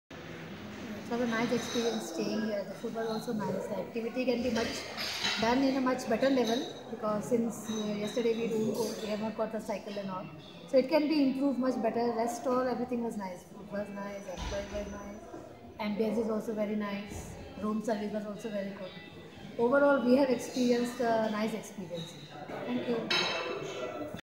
Mrs. Vikas and family from Gujarat
Mrs. Vikas and family from Gujarat shares their experience of an amazing holiday.